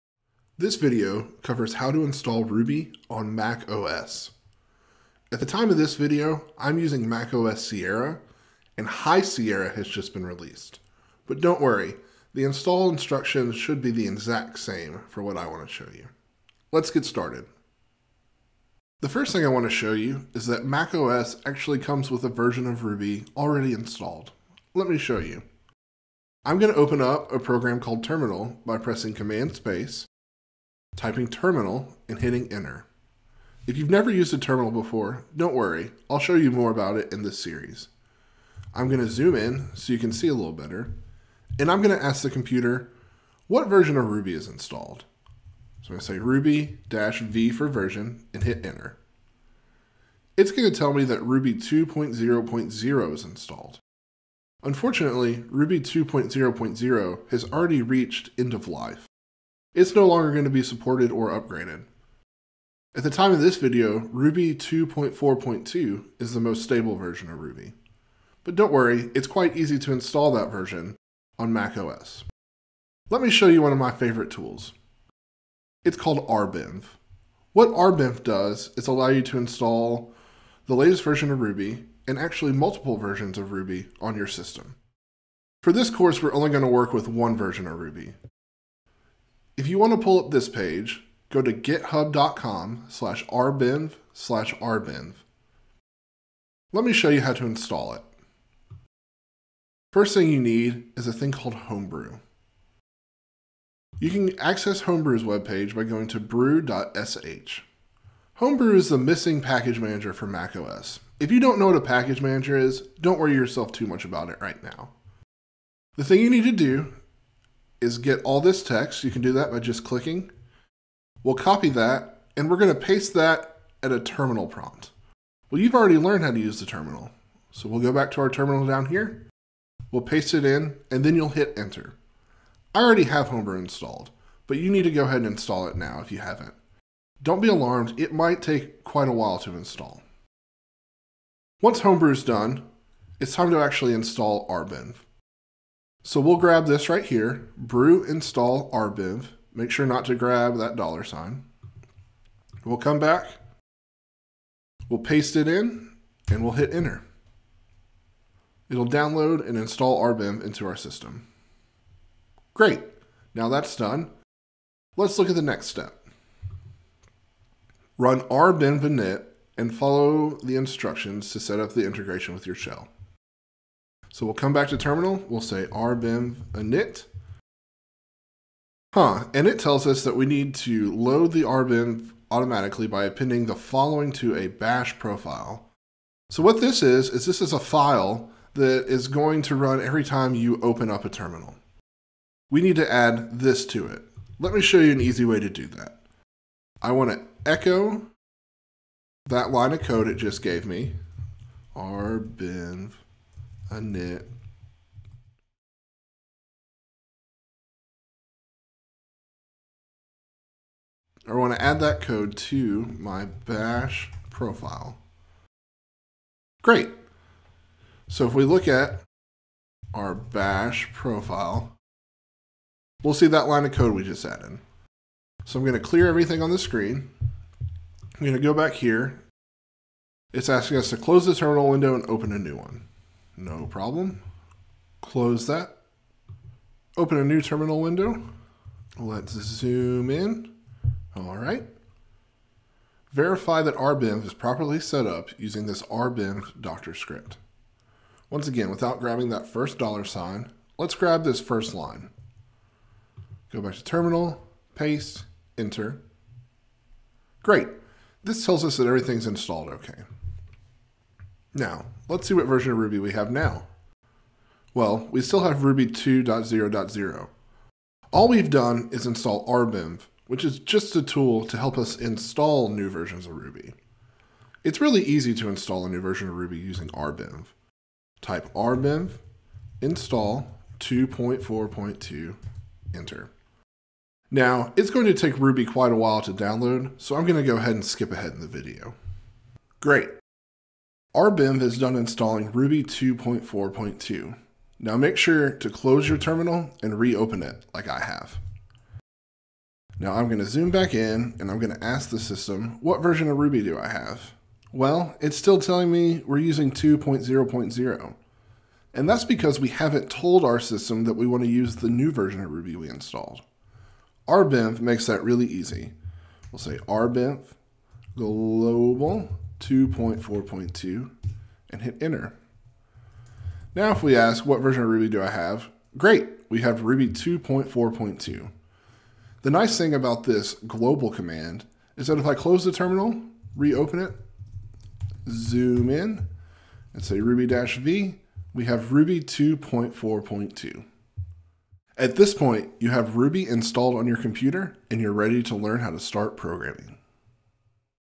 Install Ruby on macOS with Rbenv
This video teaches you how to install the latest version of Ruby on your macOS computer. Using a tool called rbenv makes installing the latest (and multiple) versions of Ruby a breeze.